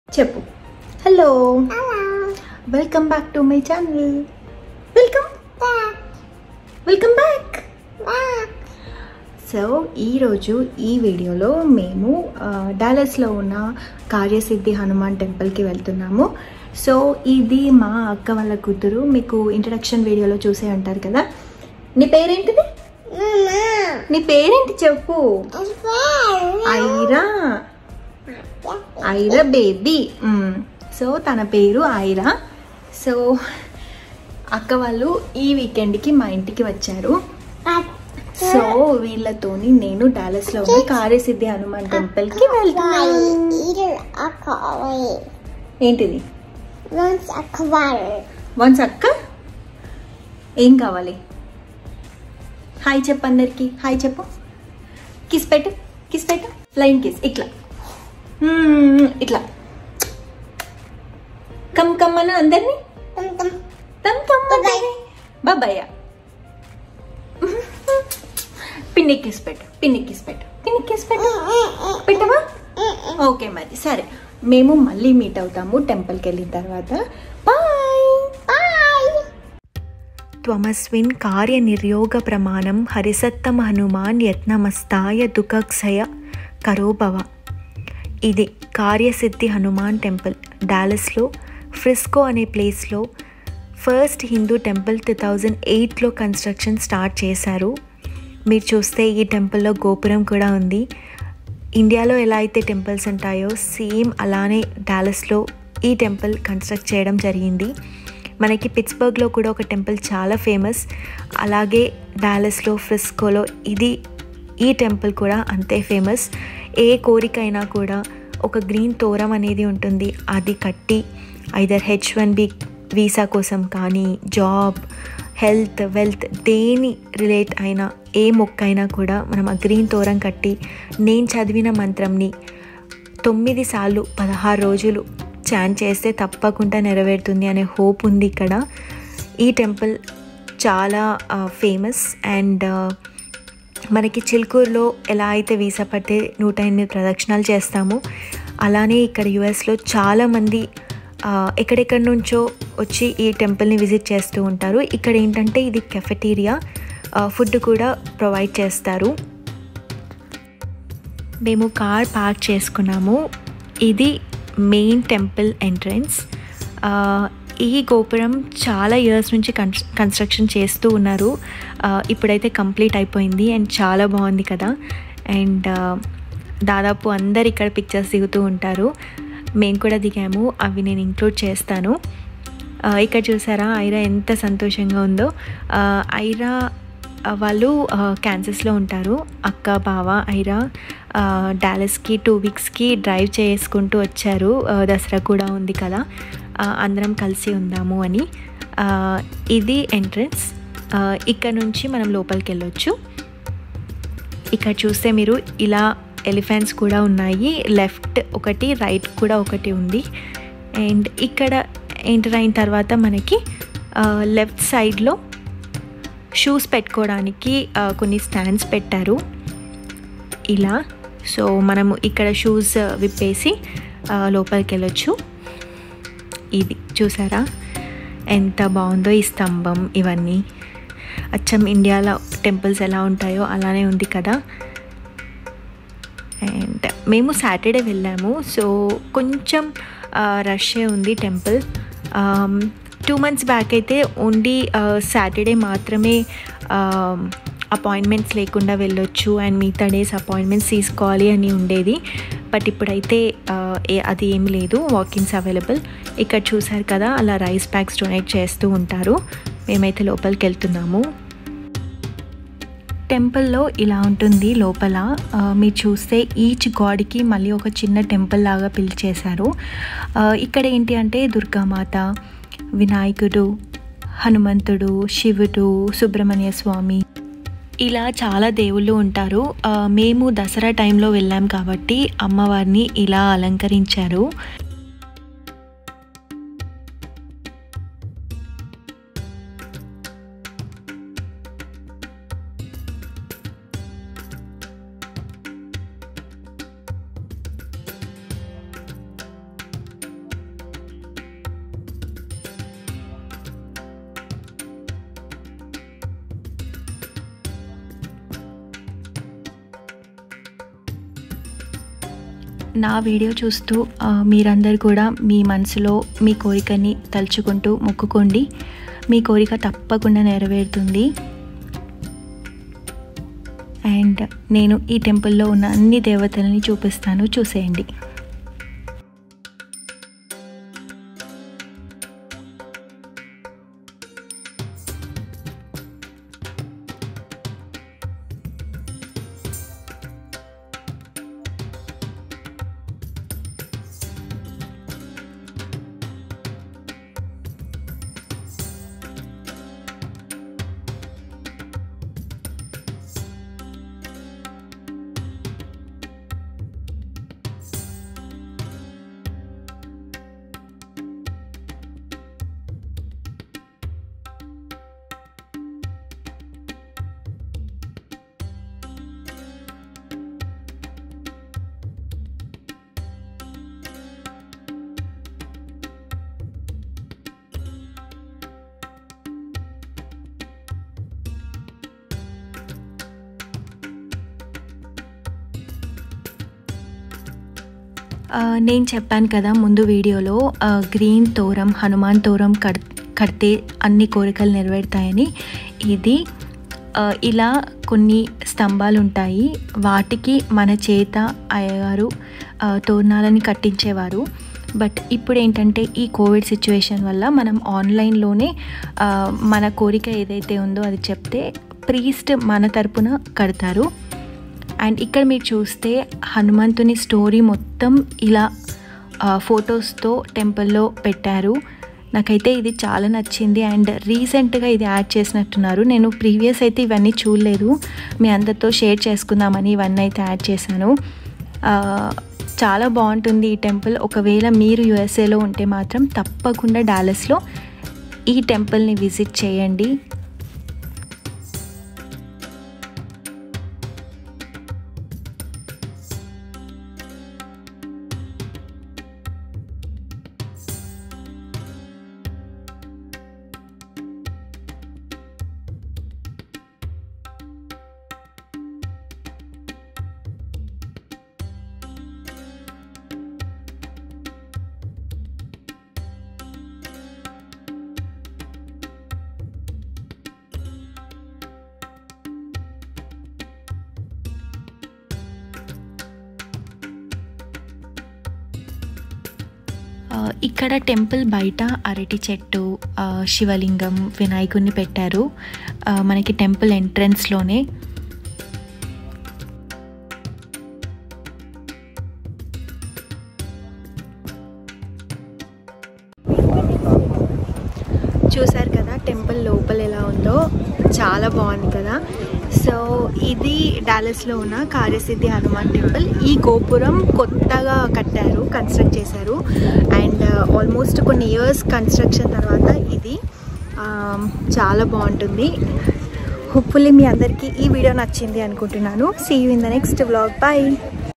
Karya Siddhi Hanuman Temple Vlog || Telugu Vlogs In USA
Hi Everyone,

In today’s video I have showed you all a famous temple in Dallas.Thank you for taking time to watch this video.

Thanks